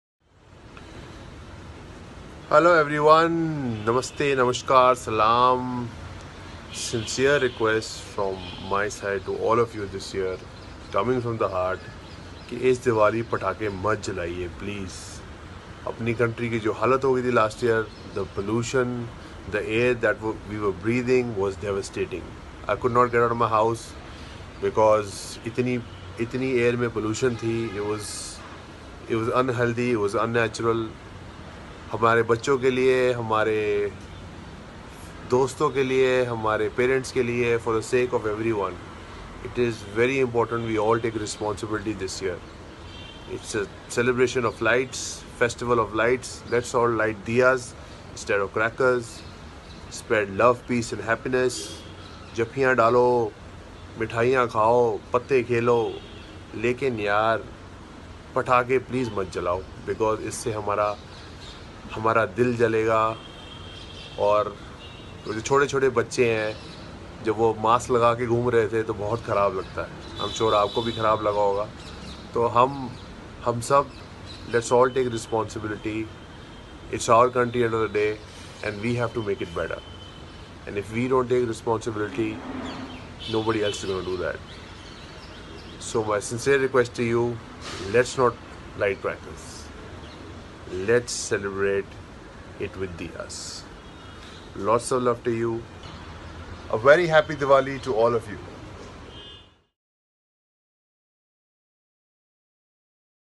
युवराज सिंग सांगतोय इको फ्रेंडली दिवाळी विषयी : Eco friendly Diwali message from Yuvraj Singh
आपल्या देशात दिवसेंदिवस प्रदूषणामध्ये वाढ होत आहे. गेल्या वर्षी आपल्या देशाची अवस्था प्रदूषणामुळे बिकट झाली होती.  याच पार्श्वभूमीवर क्रिकेटपटू युवराज  सिंगने देशवासियांना यंदाची दिवाळी  इको फ्रेंडली साजरी करण्याचे आवाहन केले आहे. या दिवाळीत फटाके वाजवू नये. आपले  फ्रेंड्स, पालक, लहान मुले यांची काळजी घेणे आपल्या  प्रत्येकाची जबाबदारी आहे. दिवाळी हा दिव्यांचा सण आहे. एकमेकांना भेट,  मिठाई वाटा, पण  फटाके वाजवू नका असे युवी म्हणत आहे.